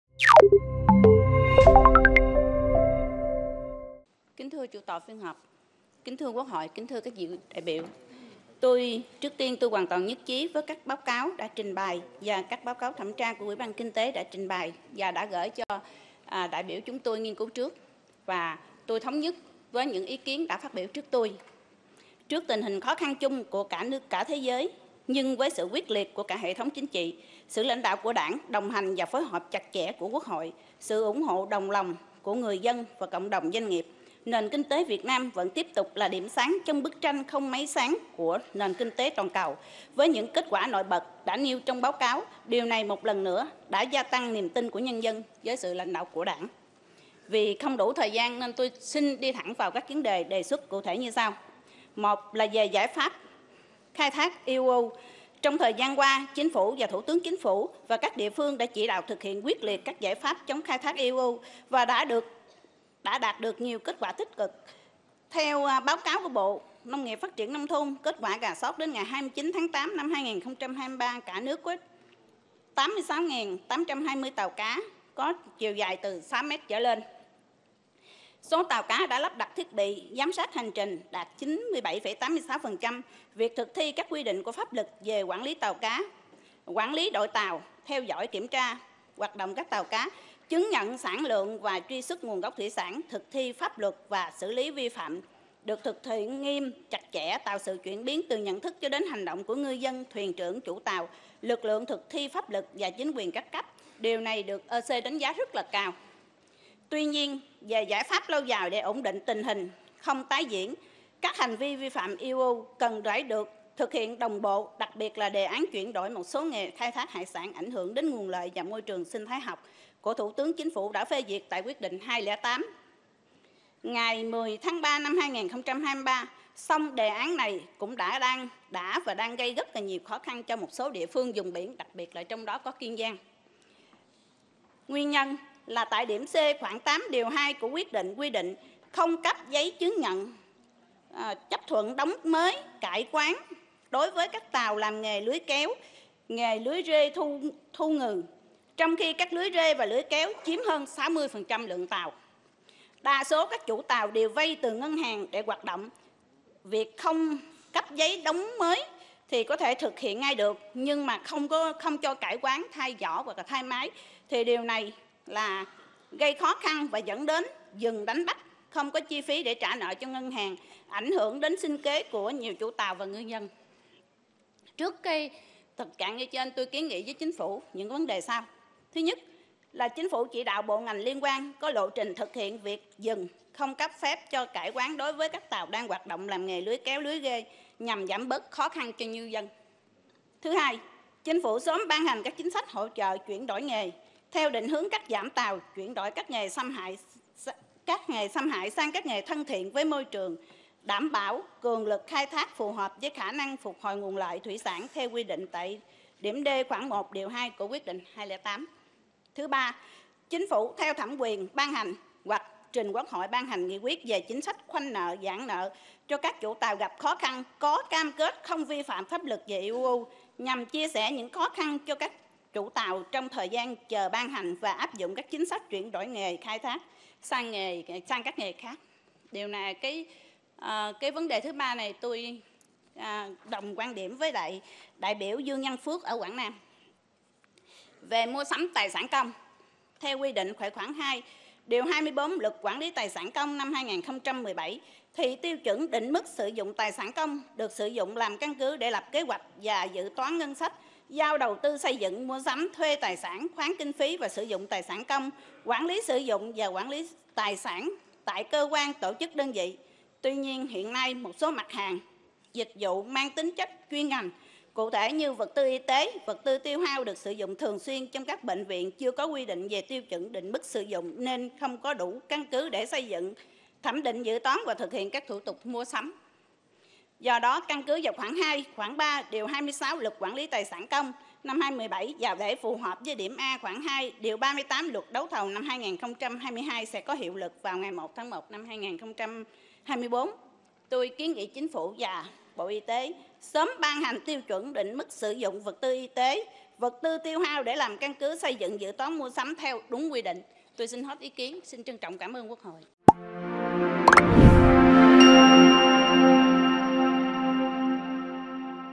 Đại biểu Lý Anh Thư: Cần sớm ban hành các chính sách hỗ trợ chuyển đổi nghề
Truyền hình Quốc hội Việt Nam | Tiếp tục chương trình làm việc kỳ họp thứ 6, Quốc hội khóa XV, thảo luận chiều 31/10 về tình hình kinh tế - xã hội, đại biểu Lý Anh Thư - Đoàn ĐBQH tỉnh Kiên Giang đề nghị sớm ban hành các chính sách hỗ trợ chuyển đổi nghề theo định hướng cắt giảm tàu, chuyển đổi các nghề xâm hại sang các nghề thân thiện với môi trường đảm bảo cường lực khai thác phù hợp với khả năng phục hồi nguồn lợi thủy sản.